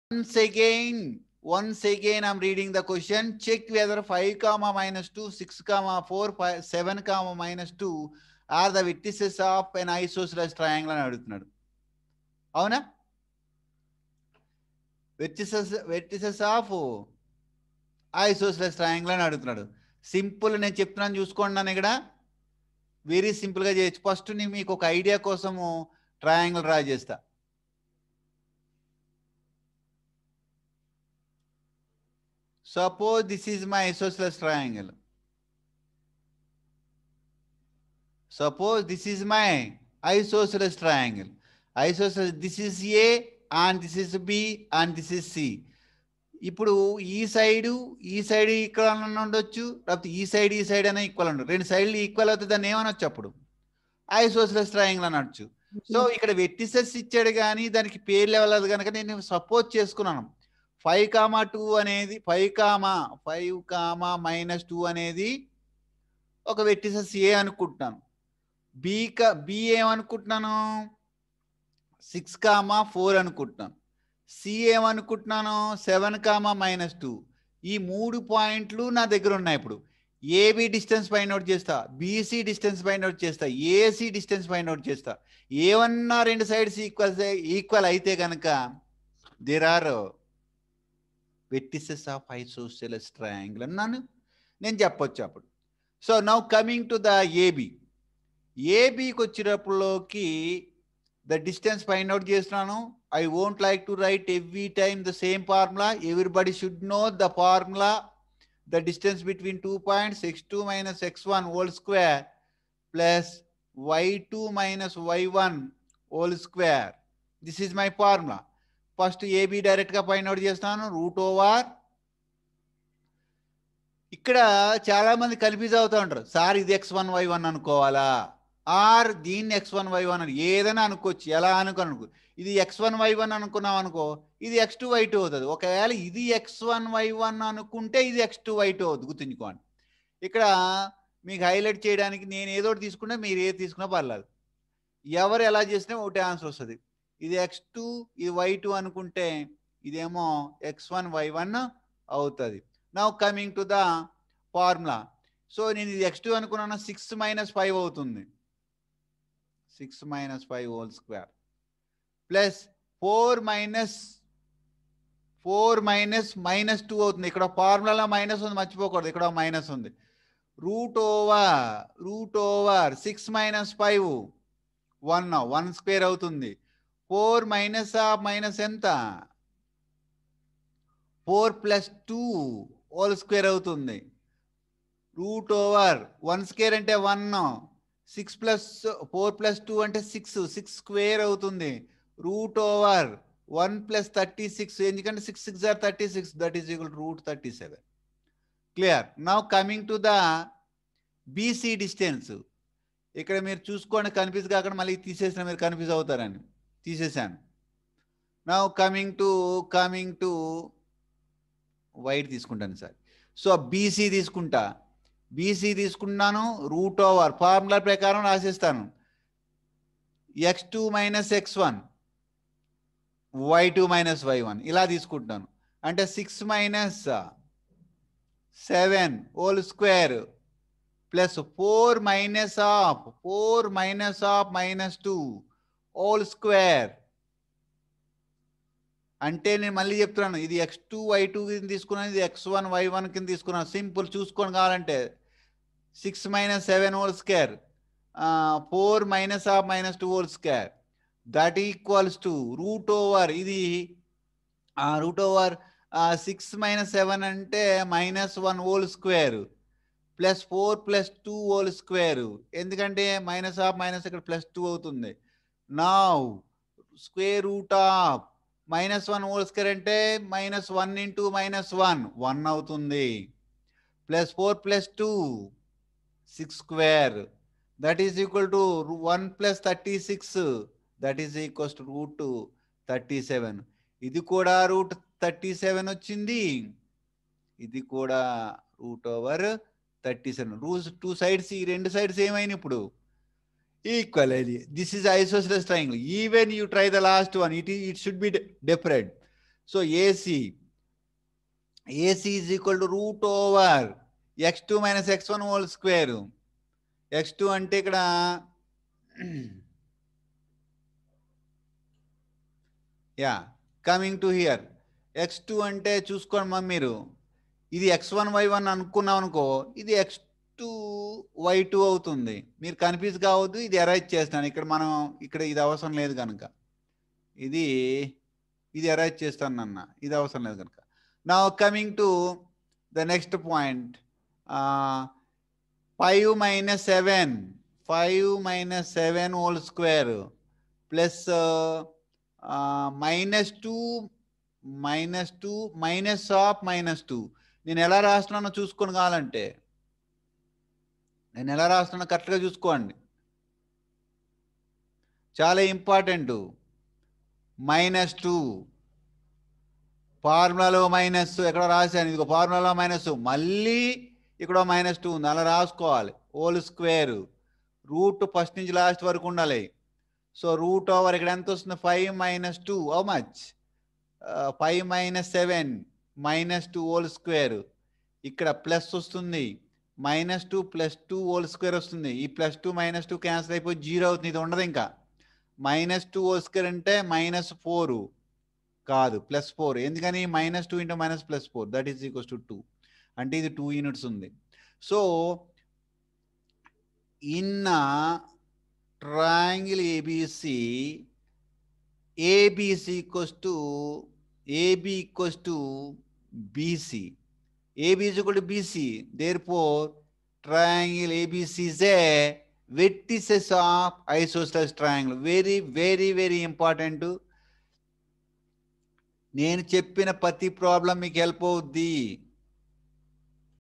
Class 10 Coordinate Geometry exercise - 7.1
4. Check whether (5, -2), (6, 4) and (7, -2) are the vertices of an isosceles triangle.